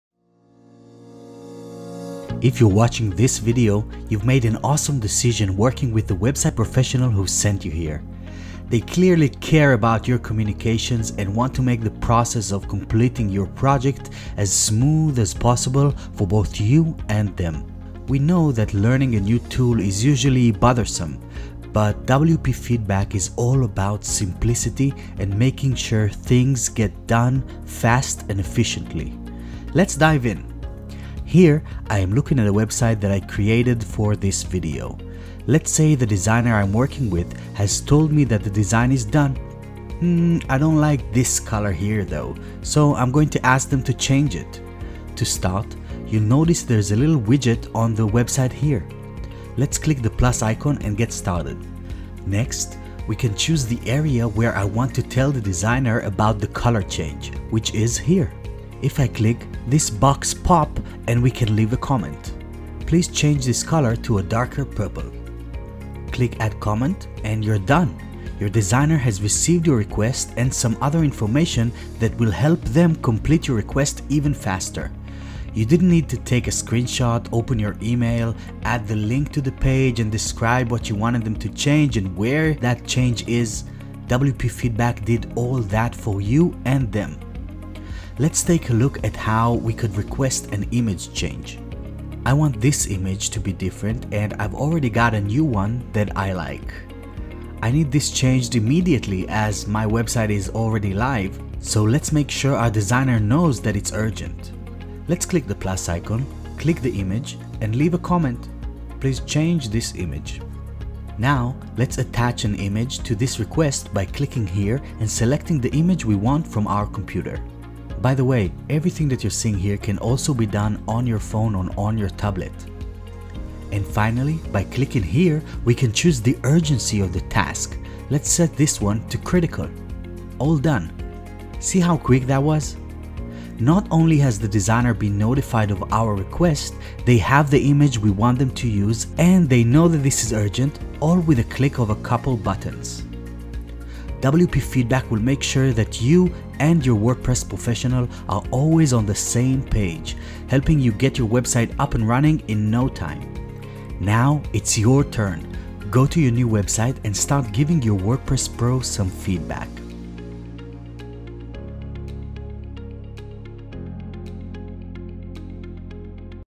Atarim Plugin #12 Send this to your client
If you are reading this, you've made an awesome choice working with the WordPress professional that sent you here. We will show you how to use WP FeedBack PRO to make sure your project is completed efficiently!

Do you want to see how much time and money YOU could save using WP FeedBack PRO?